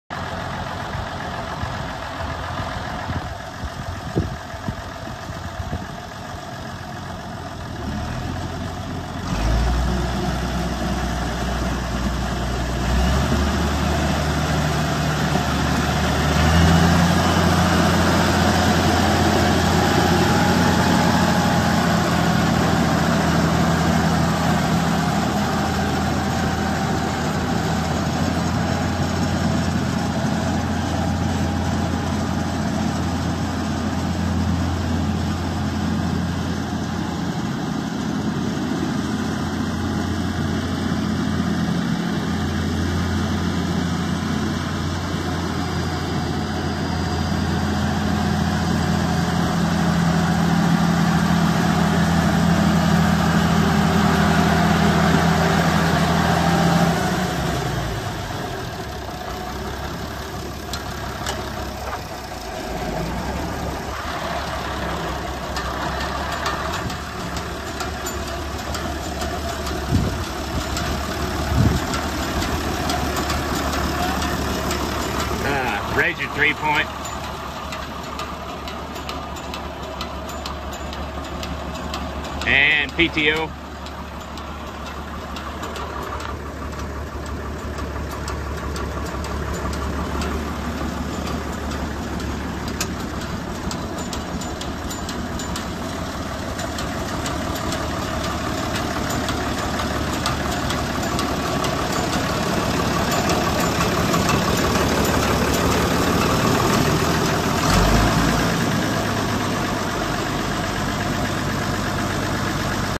INTERNATIONAL 1456 For Sale
International Farmall 1456 turbo with 6329 hours. Lots of work done. New front tires, new alternator, new batteries, new brake pedal assembly, remotes rebuilt. 540/1000 PTO. 6 front weights. 2 rear wheel weights. 2 sets of remotes.